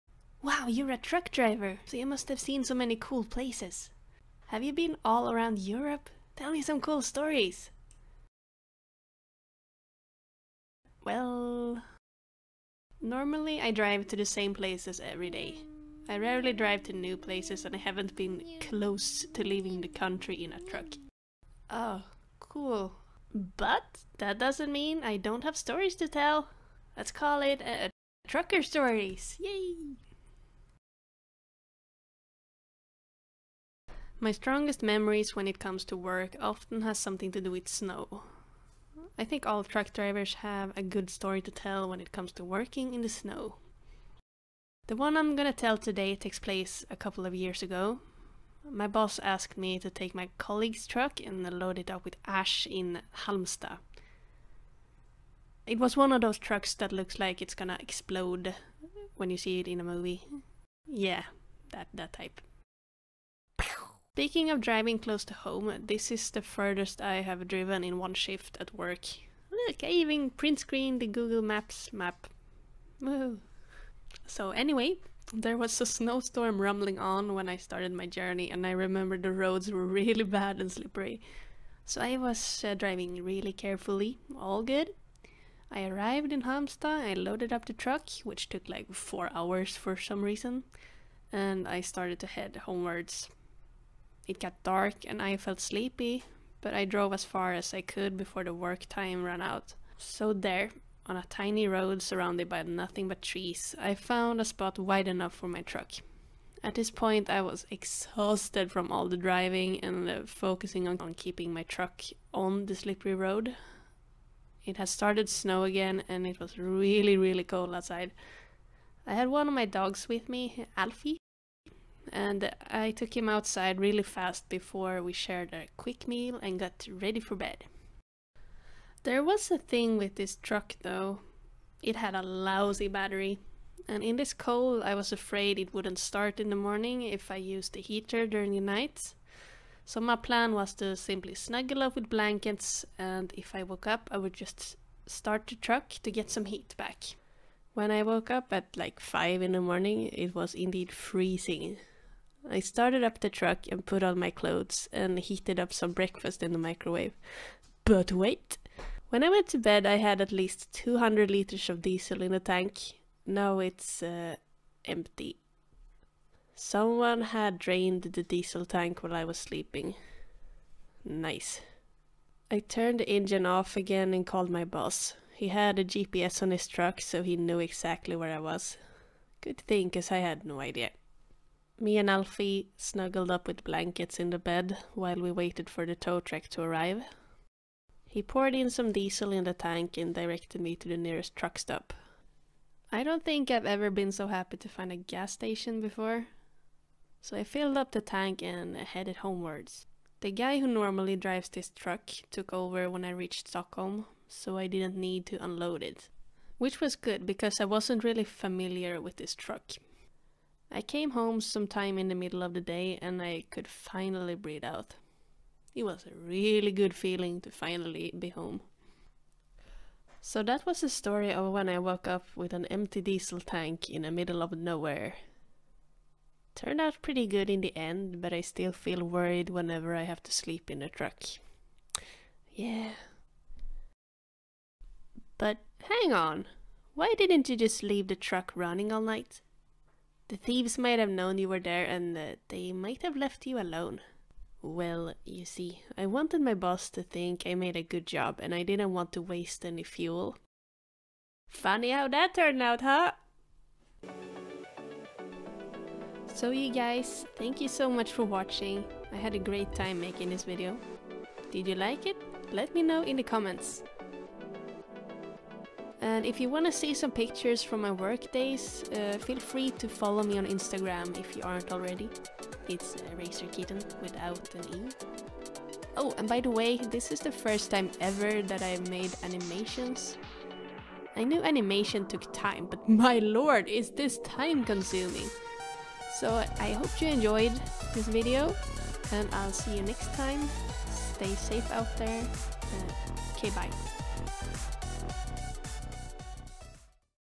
Empty Diesel Tank - Trucker Stories #1
That time when I ended up with an empty diesel tank in the middle of nowhere.